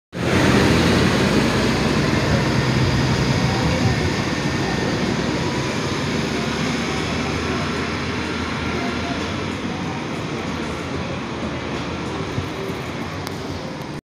CTtransit 2008 New Flyer D40LF #801
Engine: 8.9 liter Cummins ISL 280 HP EPA 2007

Transmission: Allison B400R 6 speed Gen IV

Axles: MAN

Taken: 9/2/2022